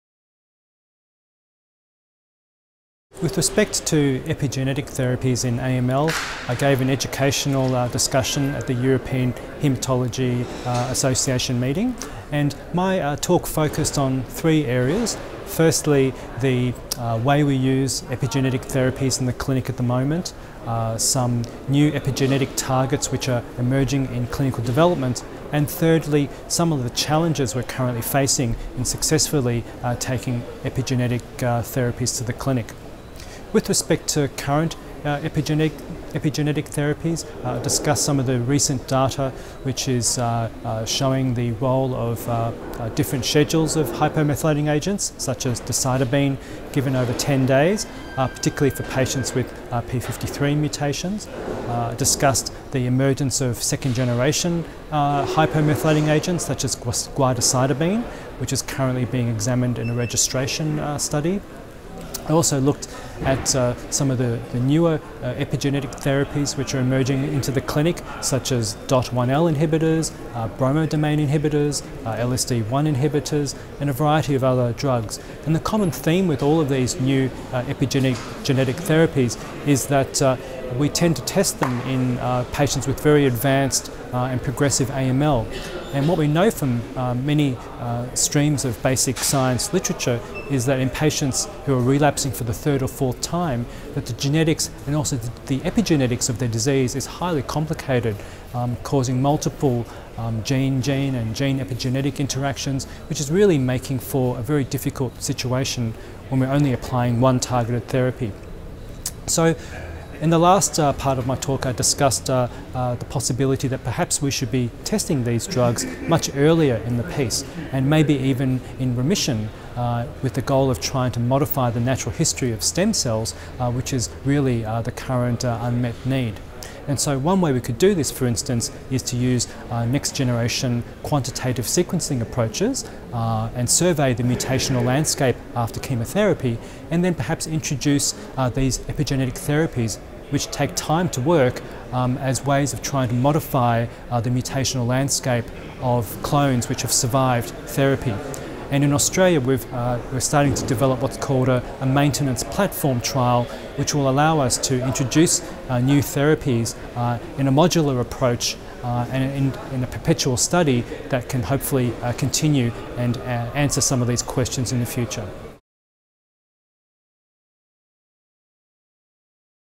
Epigenetic therapies in AML: current and future landscape
Epigenetic therapies are constantly developing in the field of acute myeloid leukemia (AML). In this video, recorded at the 23rd Congress of the European Hematological Association (EHA) 2018, held in Stockholm, Sweden, Andrew Wei, MBBS, PhD, of Monash University, Melbourne, Australia, gives an overview of the current landscape of epigenetic therapies for AML, as well as his predictions for the future directions that this field will take. He discusses the utility of hypomethylating agents, such as decitabine, for patients with TP53 mutations, as well as second generation hypomethylating agents, like guadecitabine, and newer therapies in the clinical setting. Finally, Dr Wei suggests directions for future research, including using next-generation sequencing to identify patients who would benefit from epigenetic therapies earlier in the treatment pathway.